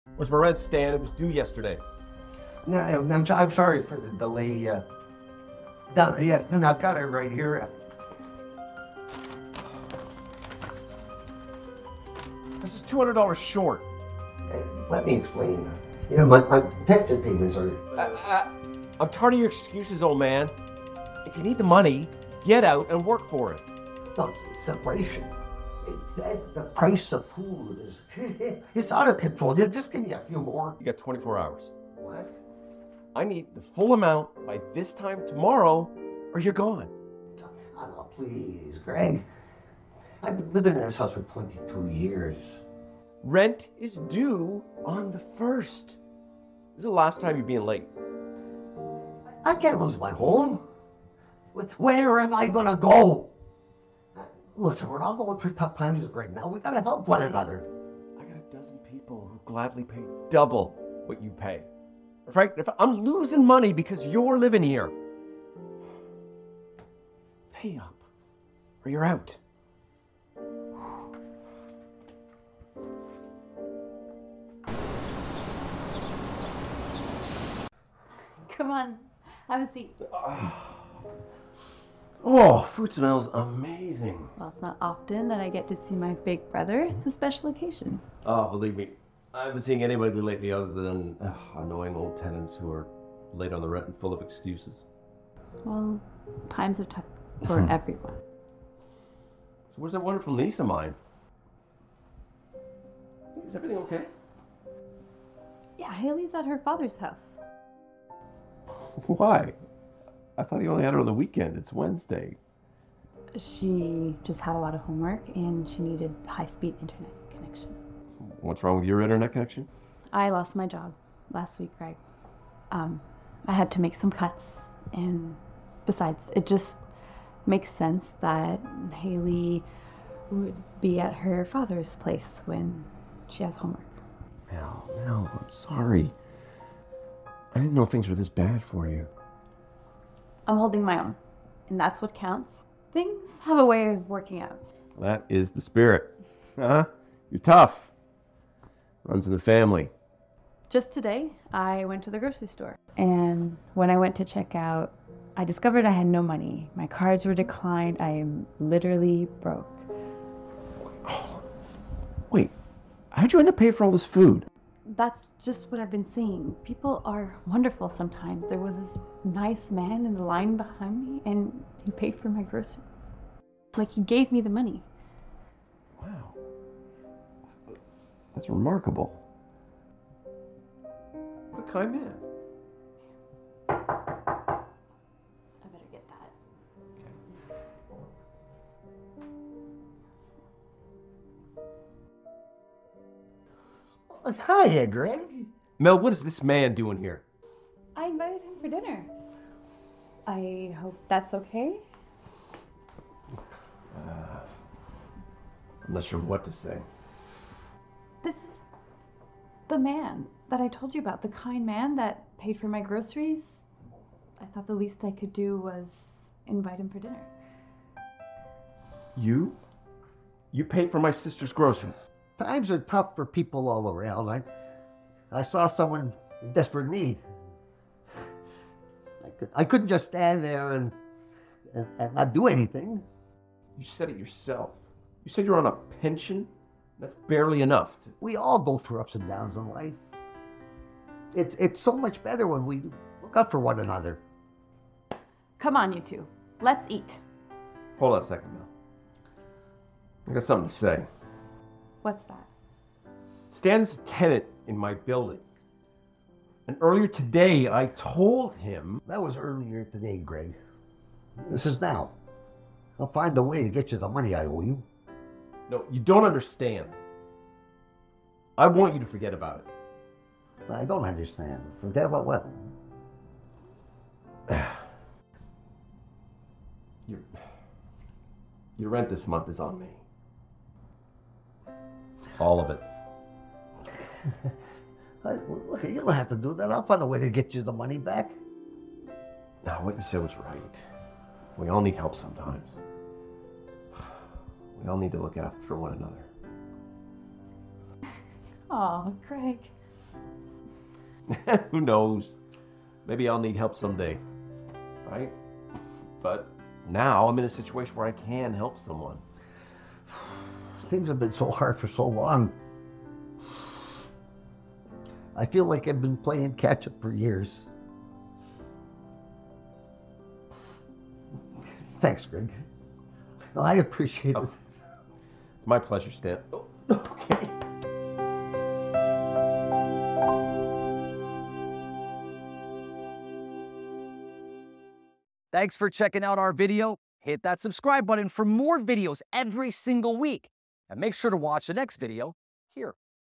Rich Landlord Evicts Poor Tenant | REIDframed Studios
Greg is a greedy landlord threatens to evict an elderly tenant named Stan from his home if he can’t come up with his late rent in the next 24 hours.

REMEMBER,  Change starts with you! So be sure to SHARE these videos so that we can help change more lives!